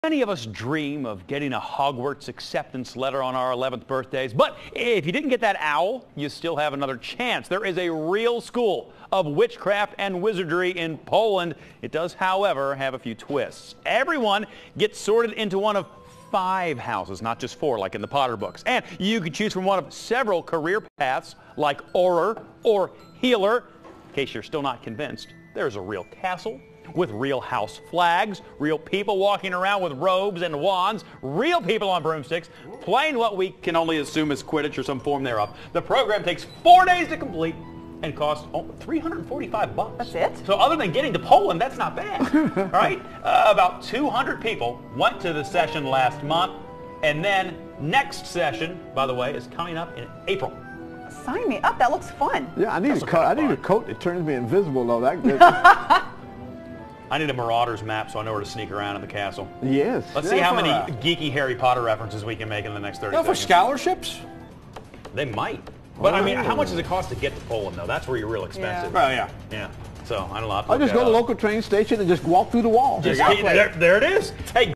The Cooler: Real school of witchcraft and wizardry
Many of us dream of getting a Hogwarts acceptance letter on our 11th birthdays. But if you didn't get that owl, you still have another chance. There is a real school of witchcraft and wizardry in Poland.

Follow Bob Herzog on Twitter @bob_herzog  and LIKE him on Facebook